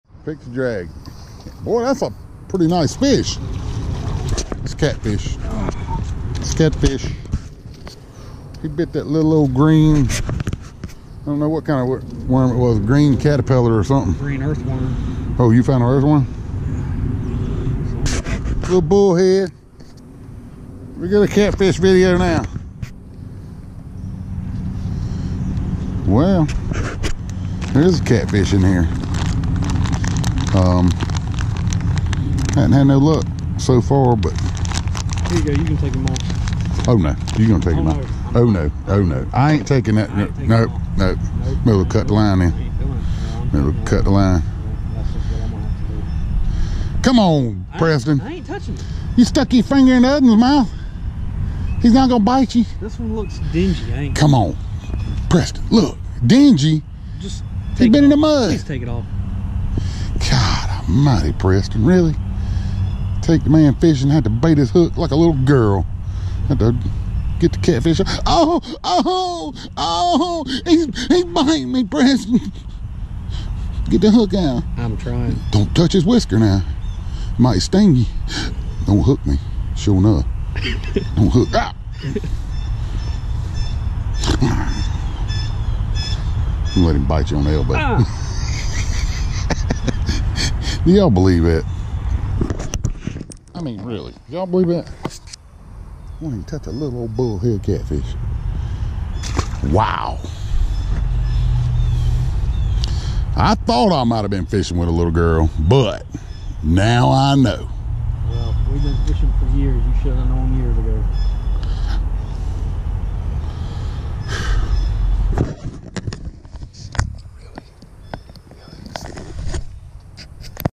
Catching Vicious Bullhead Catfish
This Guy ain’t a touching a Catfish No Matter how Small it is. These Little Bullheads Must be Vicious!!!